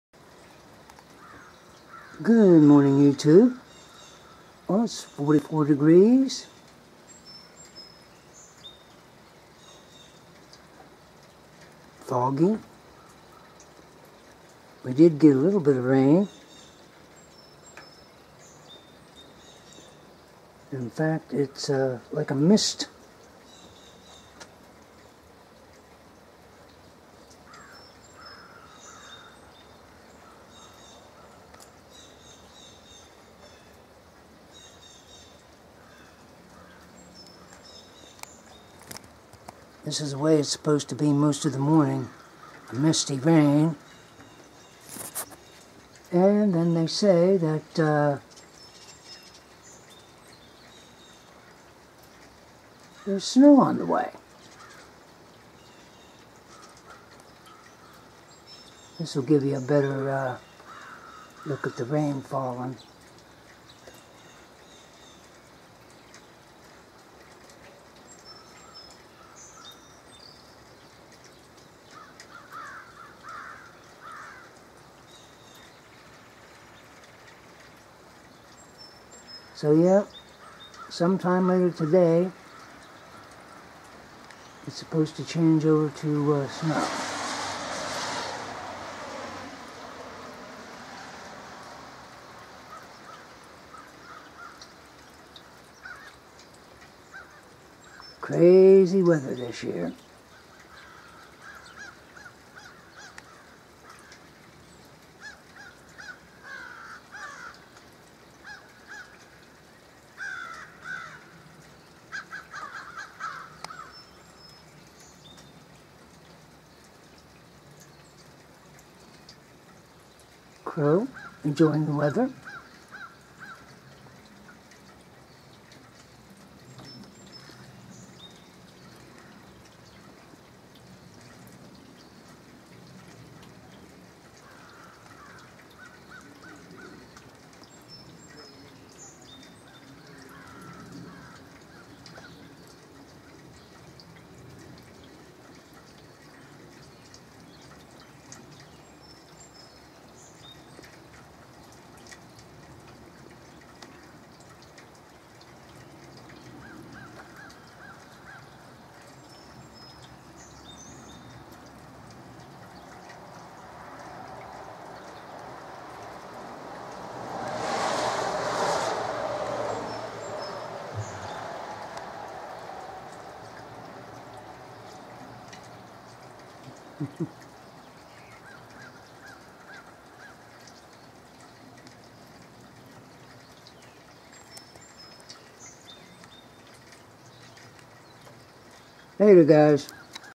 Sunday Misty Rain 2/12/2017
A foggy misty rain Sunday morning, supposeldy chaning to snow later in the day.